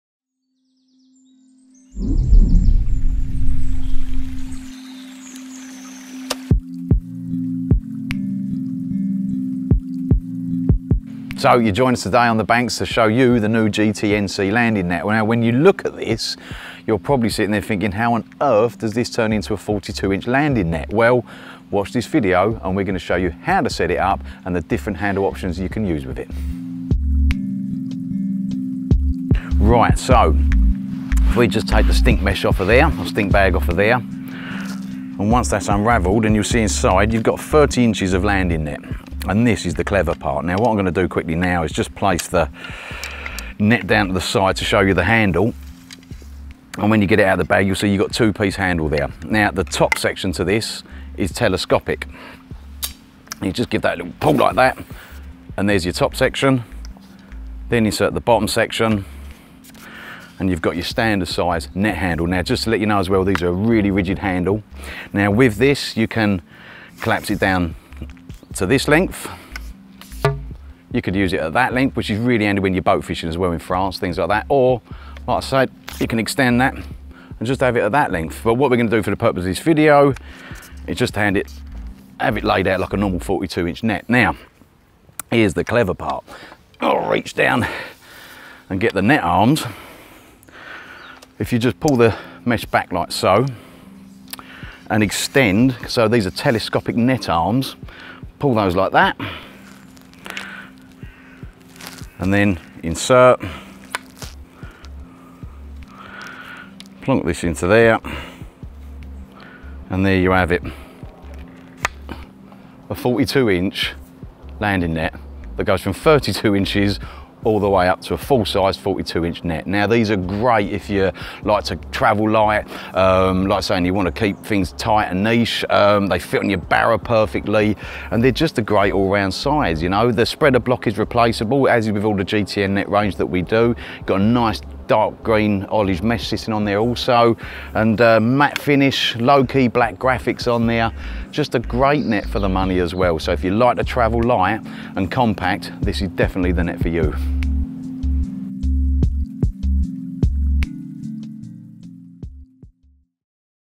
Carp fishing | GTNC Compact Landing Net | Carp fishing nets
GTNC Compact Landing Net

It folds down to just 30" long in its supplied stink bag. The 2 piece carbon handle has a telescopic top section and can be used from 76cm - 1.8m, making it ideal for boat work with just the top section. The 42" Olive Green net section has telescopic arms. The nylon spreader block is reliable, strong and can be replaced if needed.